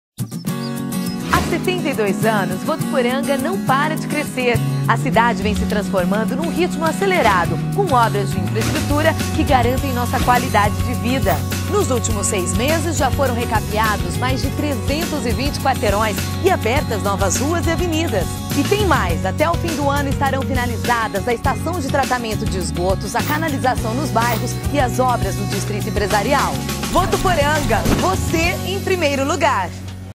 TV Prefeitura: Asfalto
Comercial da Prefeitura de Votuporanga sobre as obras em 2009